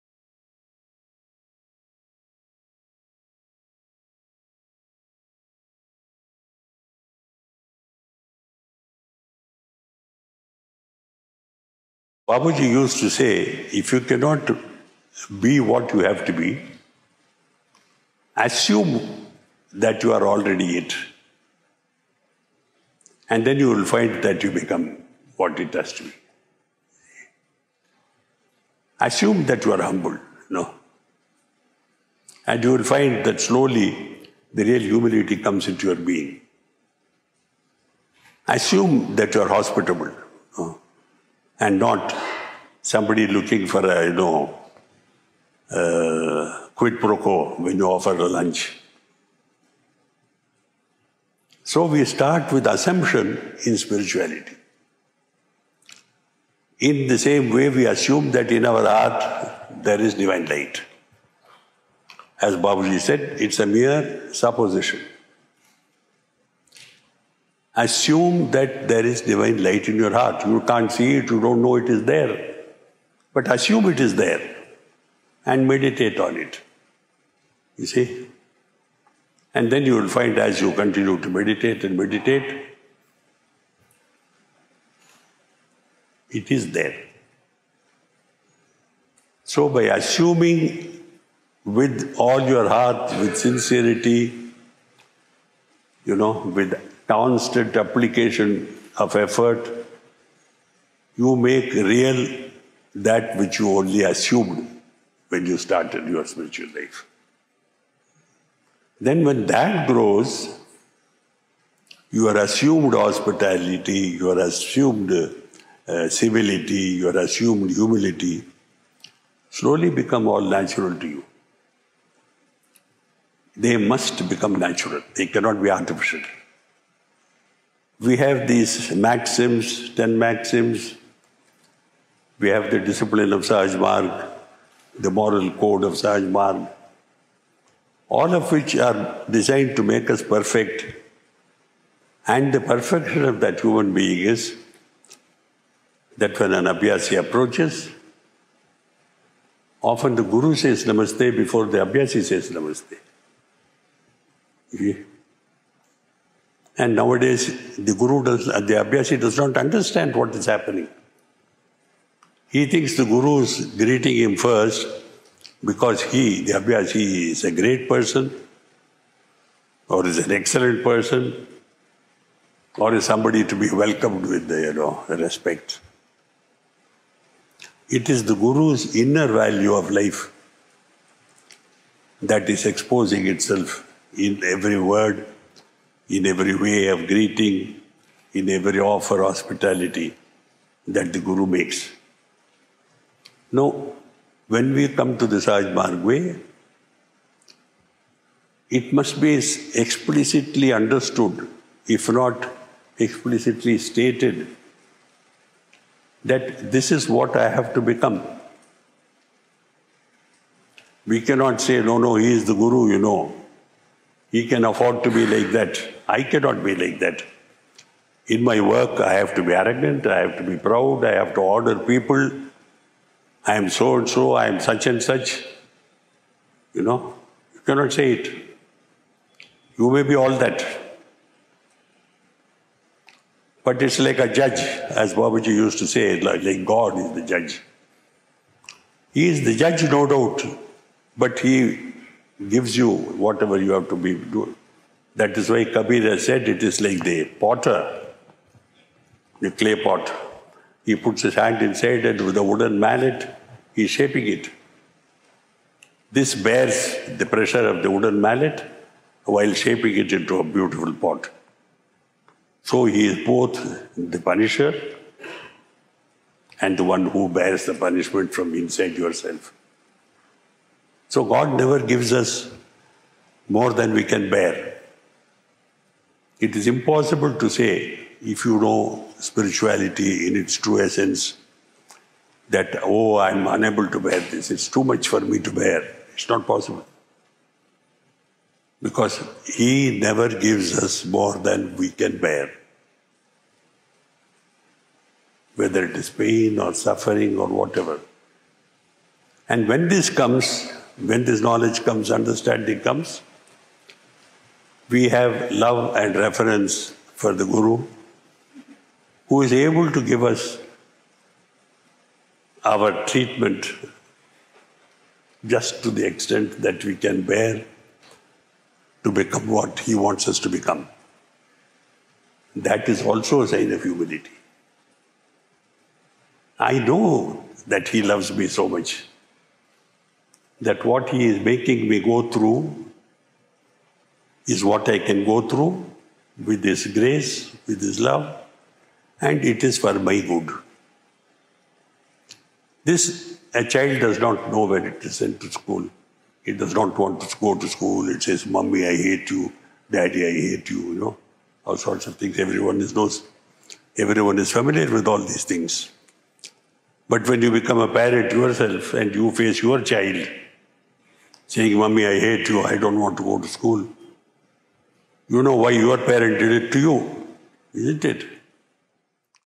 Rev: Charij If you cannot be what you have to be assume what that you are already it [Sahaj Marg]
Rev. Chariji: If you cannot be what you have to be, assume what that you are already it [Sahaj Marg SRCM]
Rev. Babuji used to say if you cannot be what you have to be, assume what that you are already it. And then you will find that you become what it has to be. Assume that you are humble and you will find that slowly the real humility comes into your being. Assume you are hospitable and not someone looking for a quid pro quo. So we start with assumption in spirituality. In the same way, we assume in our heart there is divine light. As Babuji said, it is a mere supposition. Assume that there is divine light in your heart, you can't see it and you don't know it is there, but assume it is there. And meditated on it, you see, and then you will find as you continue to meditate and meditate, it is there!

So by assuming with all your heart with sincerity you know, constant application of effort, you make real that which you only assumed when you started your spiritual life. Then when that grows, your assumed hospitality, assumed civility, your assumed humility, slowly become natural to you. They must become natural, they cannot be artificial. We have these maxims, ten maxims, the moral code of Sahaj Marg, all of which are designed to make us perfect and the perfection of that human being is that when an abhyasi approaches, often the guru says namaste before the abhyasi says namaste. And now a days, the abhyasi does not understand what is happening. He thinks that the guru is greeting him first because the abhyasi is a great person or is an excellent person or is someone to be welcomed with respect. It is the guru's inner value of life, that is exposing itself in every word, in every way of greeting, in every offer of hospitality that the guru makes. Now when we come to the Sahaj Marg Way it must be explicitly understood if not explicitly stated that this is what I have to become. We cannot say, no, no, He is the guru you know, he can afford to be like that. I cannot be like that. In my work, I have to be arrogant, I have to be proud, I have to order people. I am so-and-so. I am such-and-such. You may be all that. But its like a judge (like God). He is the judge no doubt, but he gives you whatever he has to give. That is why Kabir says it is like a potter, the clay pot, he puts his hand inside and he is shaping it. This bears the pressure while shaping it into a beautiful pot. So he is both the punisher and the who who bears the punishment from inside yourself.

So God never gives us more than we can bear. It is impossible to say, if you know spirituality in its true essence, that oh I'm unable to bear, it is too much to bear, it is not possible because He never gives us more than we can bear. Whether it is pain or suffering or whatever. And when this knowledge comes, this understanding comes, we have love and reverence for the guru who is able to give us our treatment just to the extent that we can bear to become what He wants us to become. That is also a sign of humility. I know that he loves me so much that what he is making me go through is what I can go through with his grace with his love and it is for my good. This a child does not know when it has to go to school. Does not want to go to school, mummy I hate you, daddy I hate you, all sorts of things, everyone is familiar with all these things. But when when you become a parent yourself and you face your child saying mummy I hate you, I don't want to go to school, you know why your parent did it to you. Isn't it?

Shri ParthasarathiRajagopalachari SahajMarg Shri